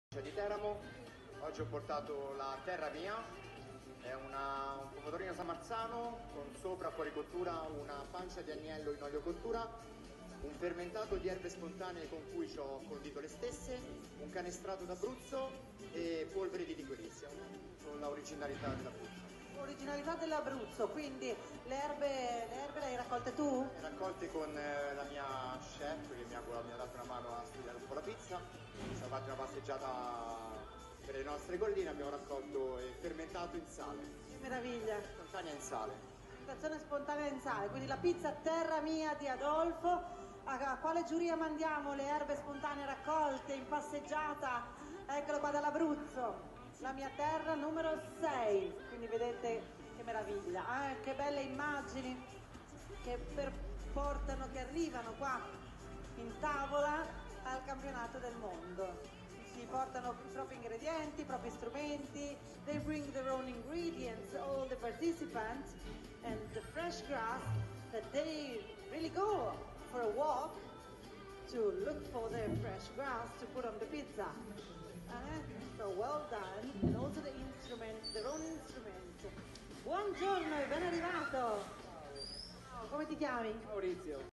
Campionato mondiale della pizza - 27° edizione - Parma 2018
Ogni anno a Parma si celebra il campionato mondiale della pizza, i concorrenti che vi partecipano provengono da tutto il mondo. In questo video mostriamo la pizza "Terra mia" presentata dal nostro pizzaiolo Adolfo! Speriamo di avervi fatto venire l'acquolina in bocca 😋

Ristorante Il Nibbio
Via Fabio Filzi 150
Villarosa (Teramo)